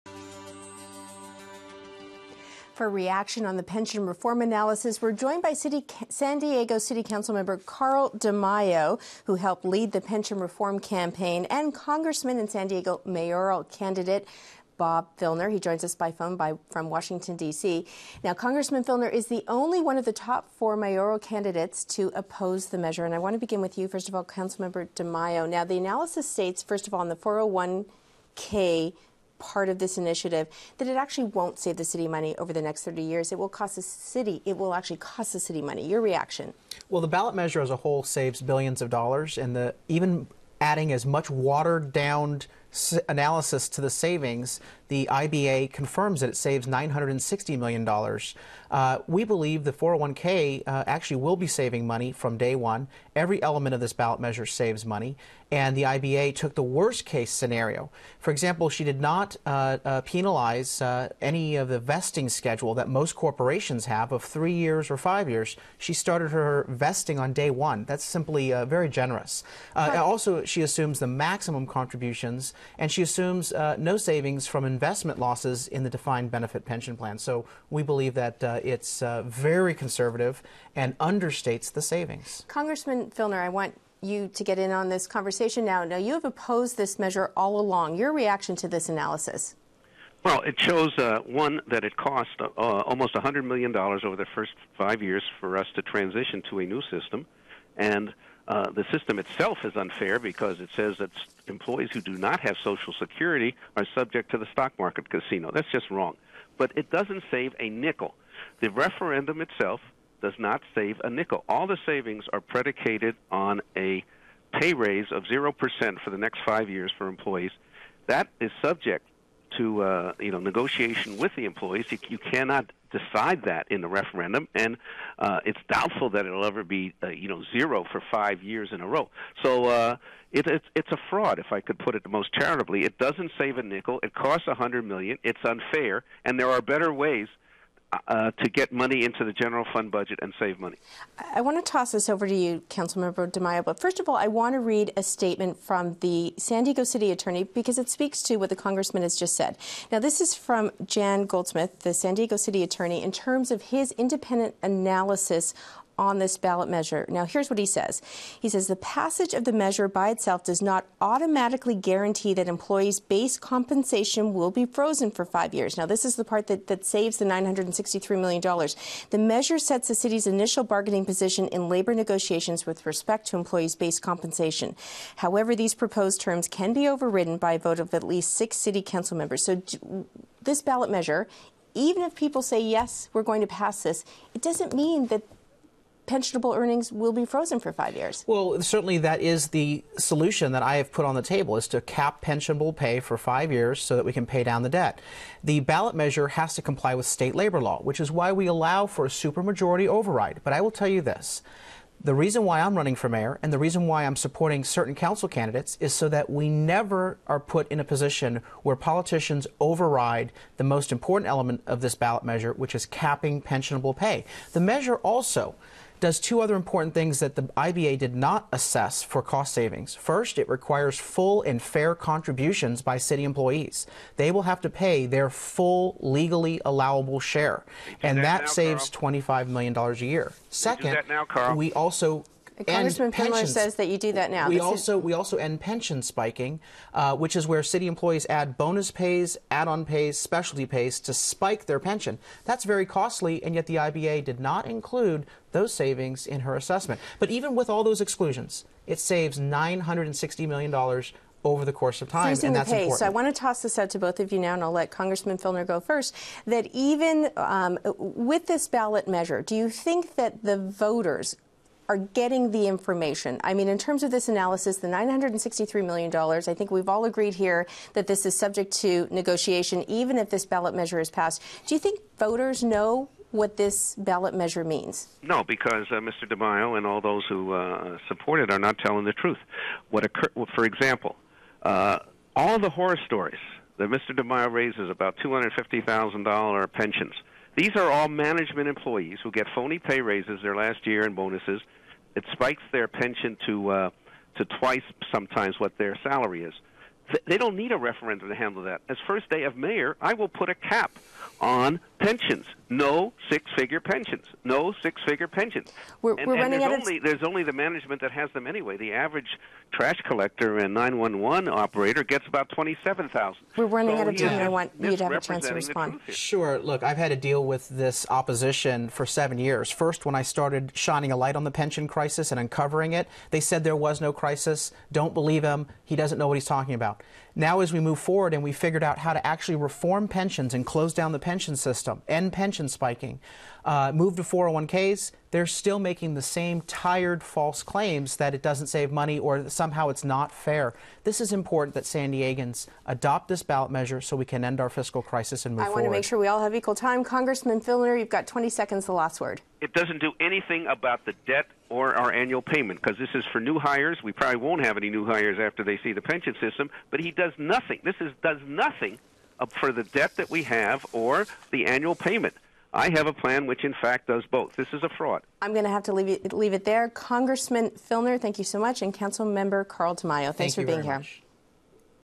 Carl DeMaio, Bob Filner on Pension Reform
Mayoral candidates Carl DeMaio and Bob Filner discuss the findings of an analysis of the pension reform ballot initiative.